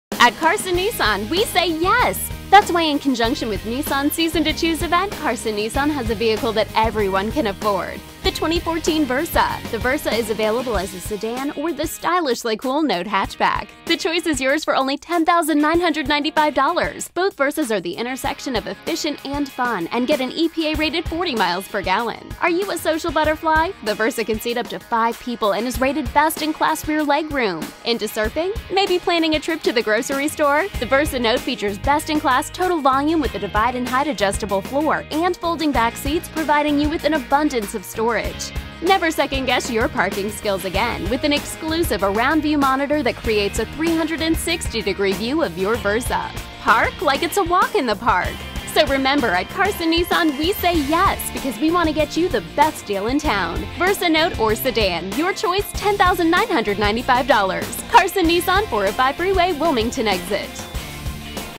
Carson Nissan 2014 Versa Sedan or Note Hatchback - $10,995
Right now at Carson Nissan, you can get a brand new 2014 Versa sedan or Note Hatchback for only $10,995! Contact Carson Nissan today!